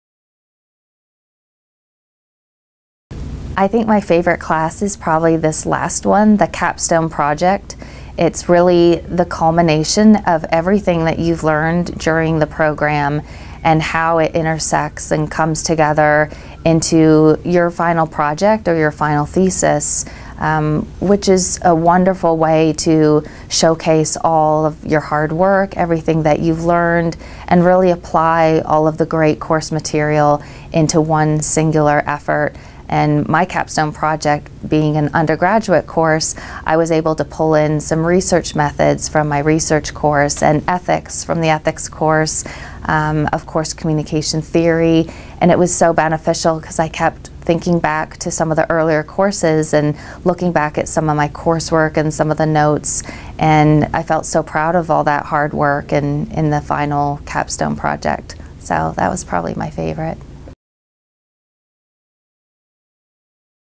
Capstone | Master's in Communication & Leadership
Master's in Communication & Leadership Alumna Carolyn Casertano explains what's involved in your final project.